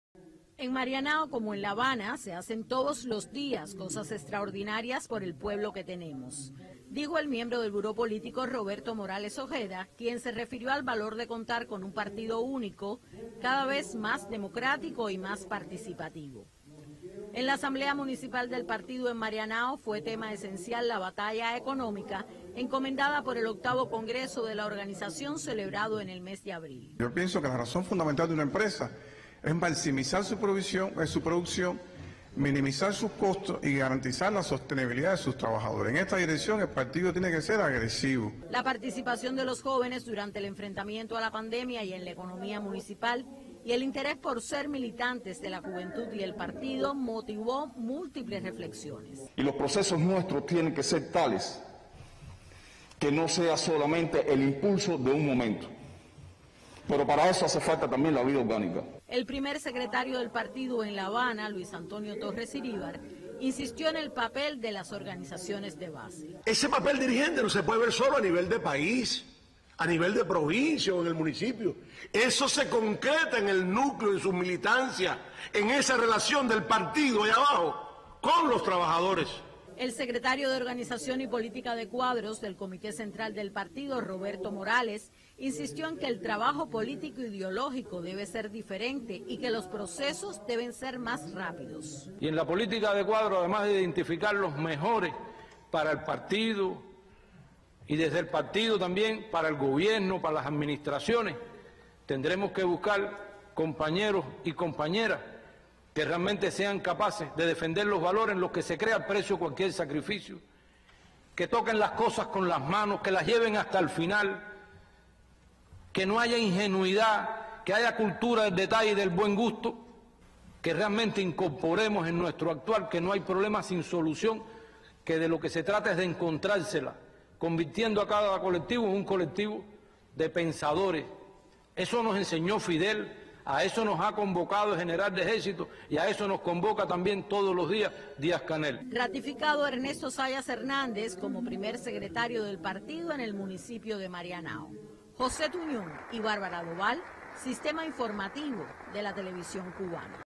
Cuba: Un partido único cada vez más democrático y participativo
En esta jornada sesionó en La Habana, la AsambleaDeBalance del Partido en el municipio de Marianao. El miembro del Buró Político y Secretario de Organización y Política de Cuadros del Comité Central del Partido Comunista de Cuba, Roberto Morales Ojeda estuvo presente en el encuentro. (Tomado del Sistema Informativo de la Televisión Cubana)